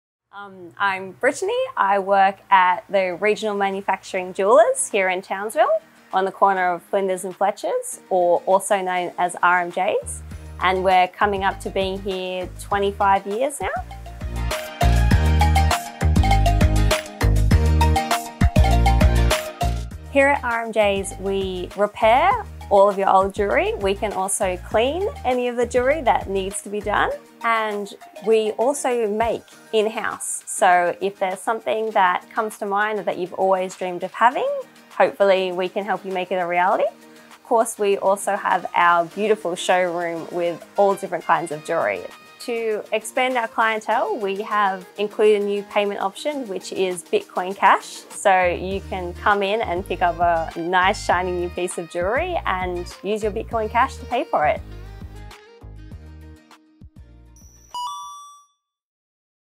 Let's celebrate one of our long-standing NQ Bitcoin Cash merchants 🎉
Ever had that unique jewellery design in your mind that you want to bring to life? RMJS Jewellers handle all kinds of requests from remodelled heirloom jewellery pieces, to ring resizing, jewellery repairs, cleans and polishes.

Pay for your bling with Bitcoin cash! 💍

🎥 DigiRyder

Chapters 📕

00:00  Intro with RMJS employee Brittany
00:12  25 years in business
00:20  The services RMJS offer
00:43  We have included Bitcoin Cash as an option to expand our clientele